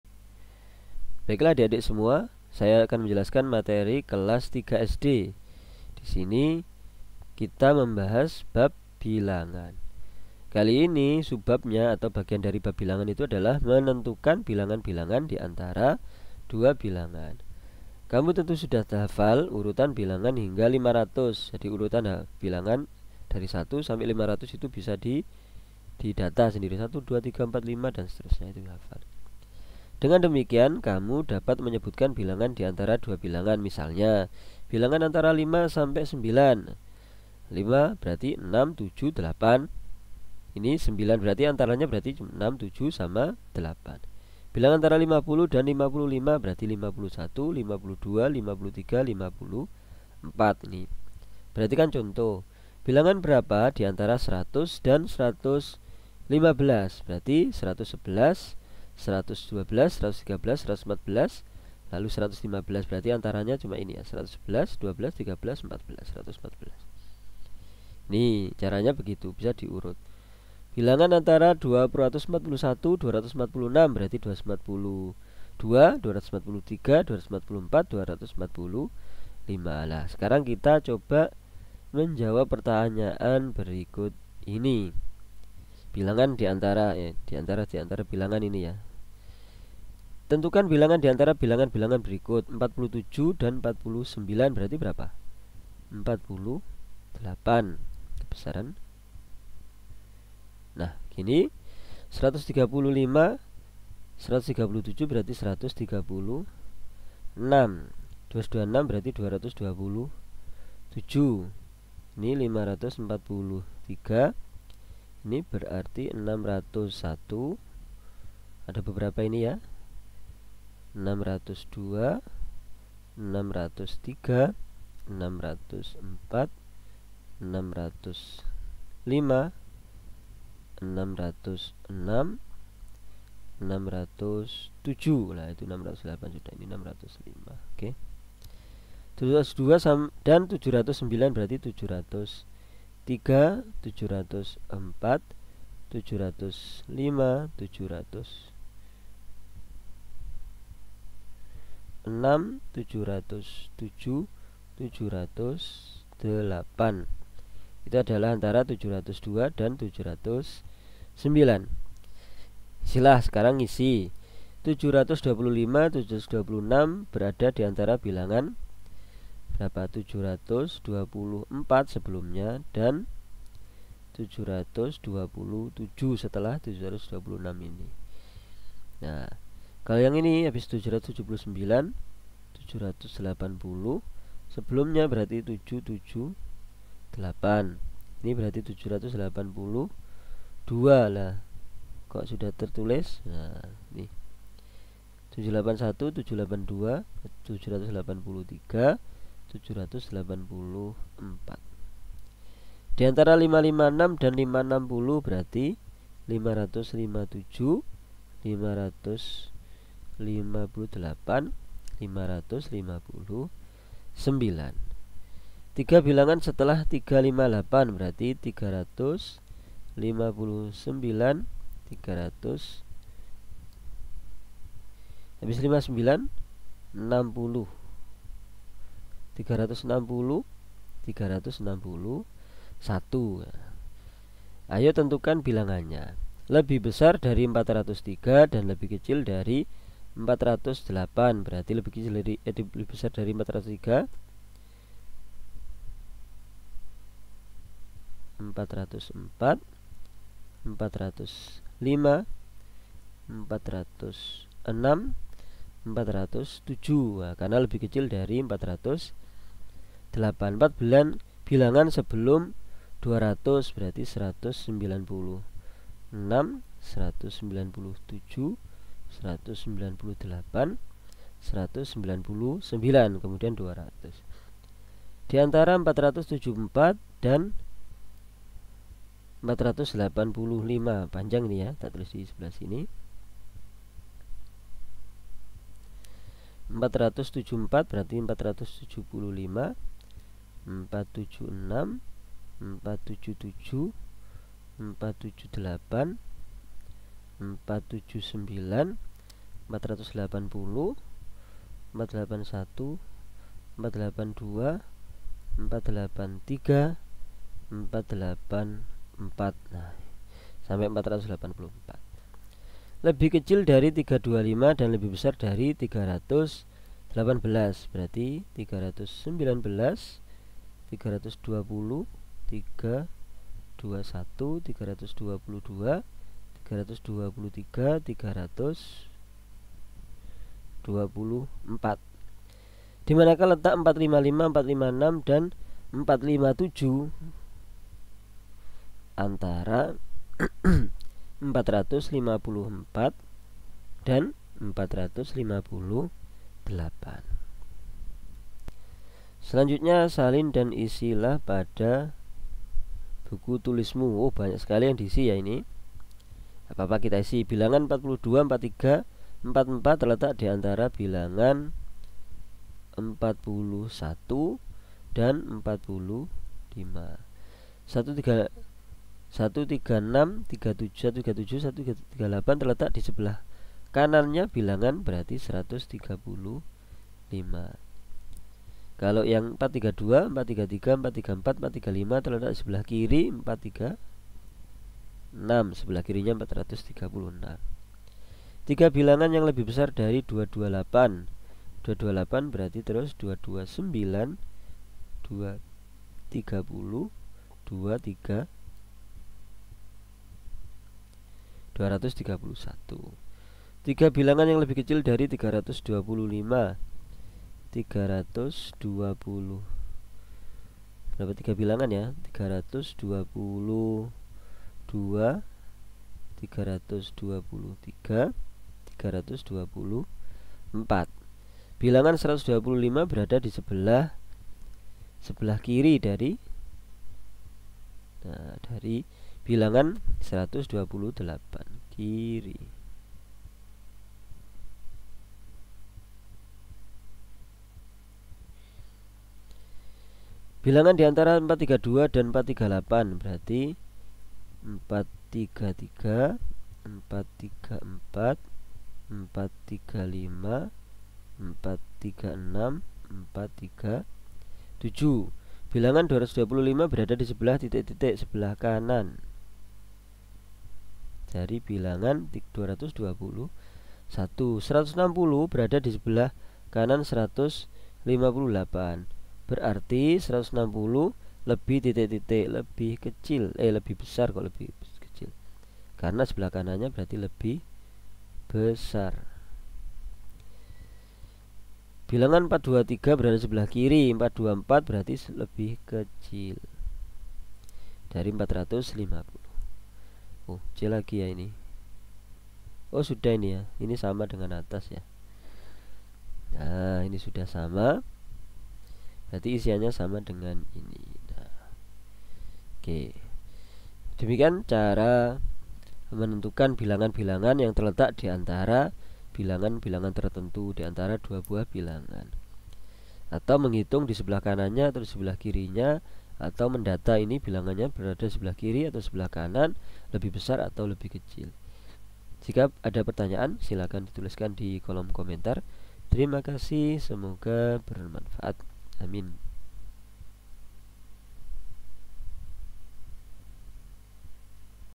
Menentukan Bilangan Bilangan di Antara Dua Bilangan - Matematika Kels 3 SD
Cara mengisi titik titik antara dua bilangan matematika kelas 3 SD.

Ingin memperdalam pemahaman Anda tentang matematika? Cek link di bawah ini untuk video pembelajaran kami!: